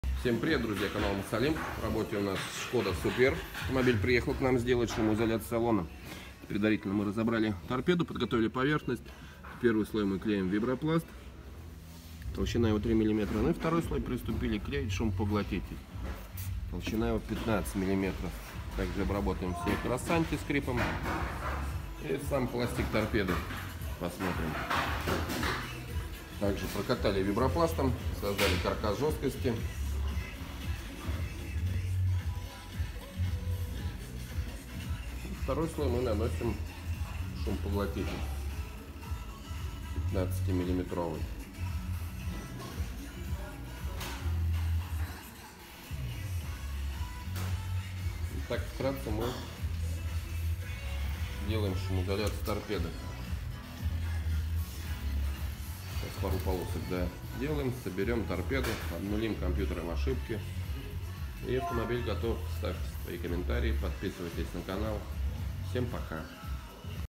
Skoda Superb антискрип торпедо. ШВИ материалами обрабатываются все пластиковые детали самой торпедо
✓ Скрипы, сверчки в торпедо? Нужно сделать шумоизоляцию пластика Skoda Superb также или ещё лучше? Консультация на сайте
Шумоизоляция и антискрип торпедо и моторного щита Skoda Superb.
Многие автовладельцы заказывает дополнительную услугу по звукоизоляции и антискрипу панели приборов и моторного щита. Что дает эта услуга? В первую очередь - гарантированное избавление от скрипа, дребезга и других сверчков в панели приборов и как дополнительный бонус - снижение шума двигателя, а на некоторых автомобилях очень и очень существенное снижение шума двигателя (зависит от автомобиля и конструкции панели приборов). Длится работа 8 часов, используются современные, эффективные шви материалы Комфортмат.